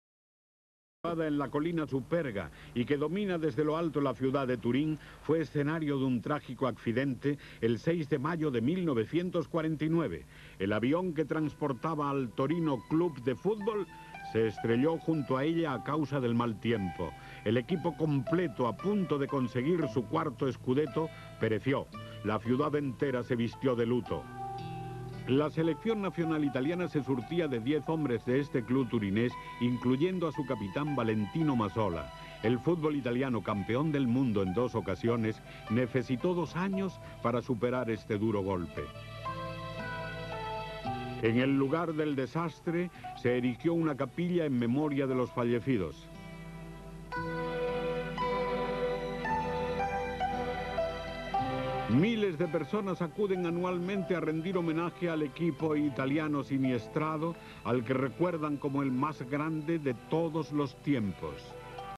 HISTORIA DEL FUTBOL Parte 14: 1949 el accidente de Superga el Gran equipo de Torino siniestrado
En 1949 el equipo italiano del Torino (base del seleccionado italiano de la epoca) muere por completo en un accidente de avion luego de la disputa de un encuentro. Aparte del costo de las vidas humanas segadas para siempre, en lo futbolistico Italia le costo recuperarse mucho tiempo en lo internacional